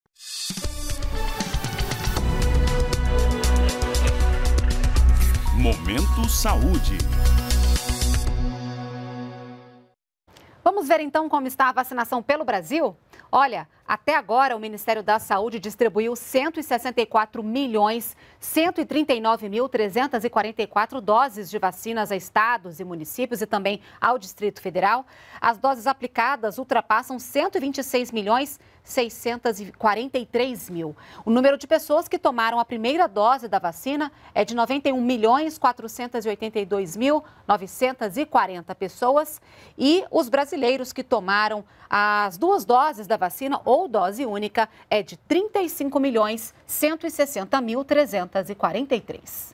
Vacinômetro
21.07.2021 – Até agora, o Ministério da Saúde já distribuiu mais de 164 milhões de doses de vacinas contra a covid-19.